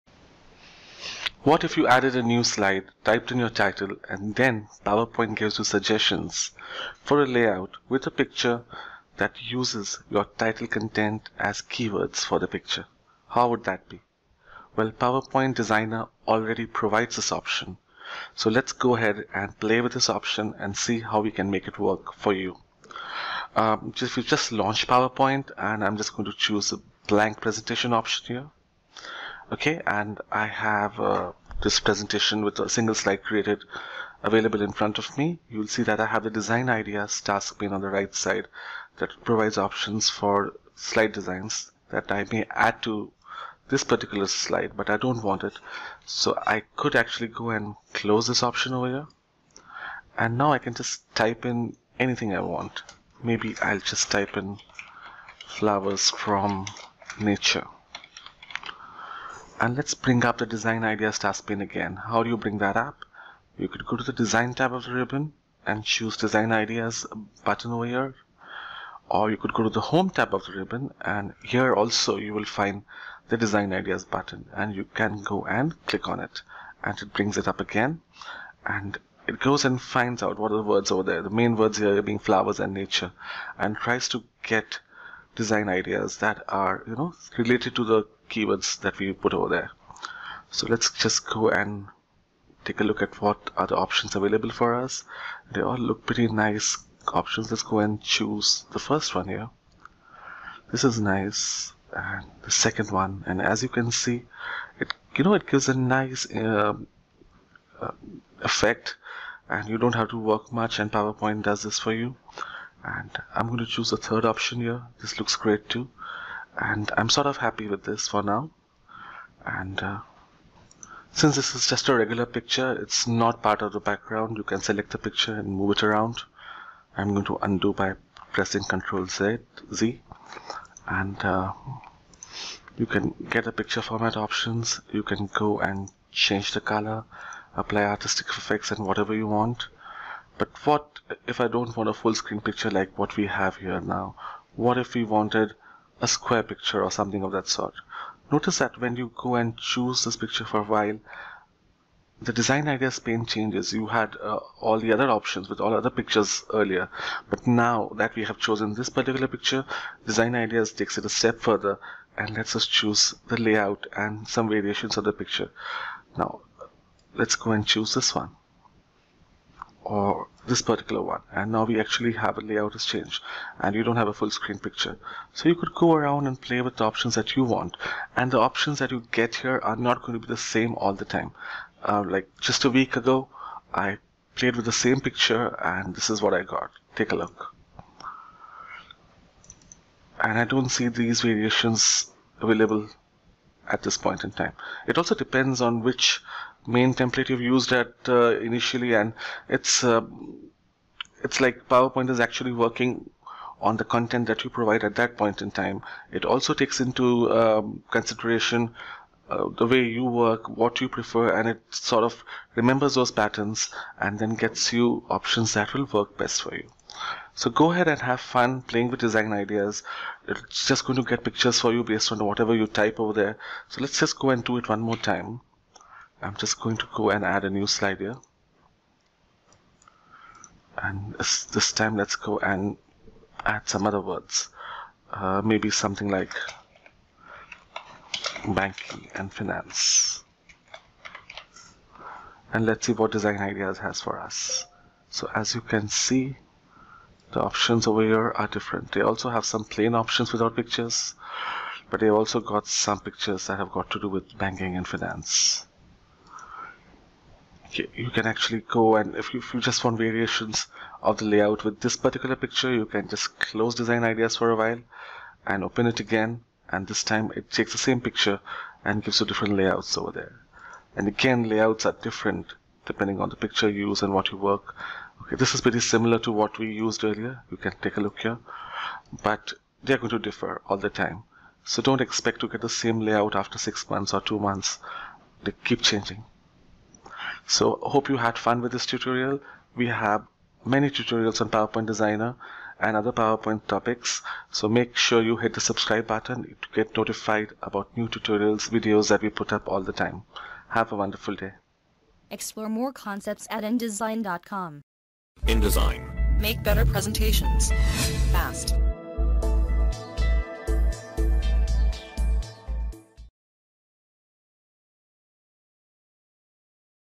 Add Photos and Layouts to Title Slides with PowerPoint Designer (06)
This video accompanies the detailed tutorial, Add Photos and Layouts to Title Slides With PowerPoint Designer in PowerPoint 365 for Windows

Add pictures and change layouts for title slides using PowerPoint Designer in PowerPoint 365 for Windows. These quickly-generated ideas can help you create better-looking slides easily.